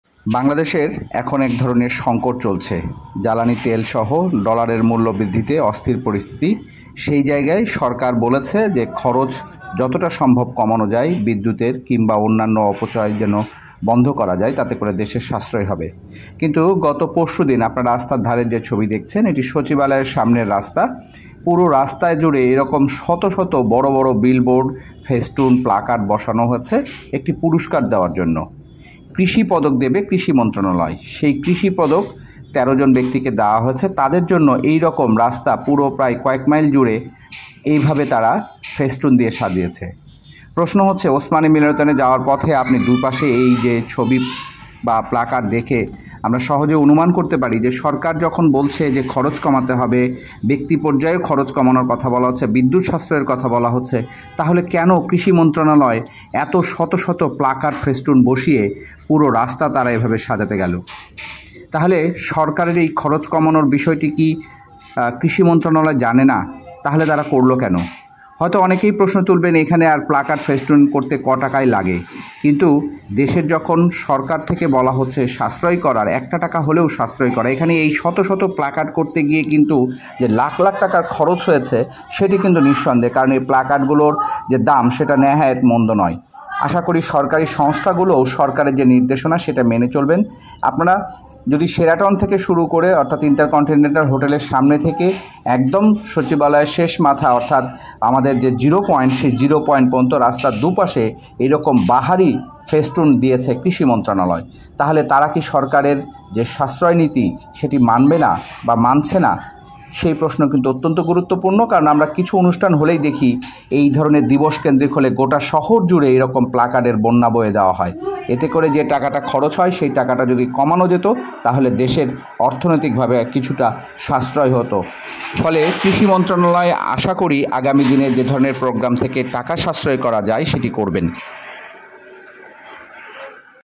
লাখ লাখ টাকার অপচয় ফেস্টুনে ! সরকারের অপচয় বন্ধের নির্দেশ মানছে না কৃষি মন্ত্রণালয়
জ্বালানিসহ বিভিন্ন খাতে খরচ কমানোর কথা বলেছে সরকার কিন্তু তা আদৌও কতটা মানছে মন্ত্রণালয়গুলো?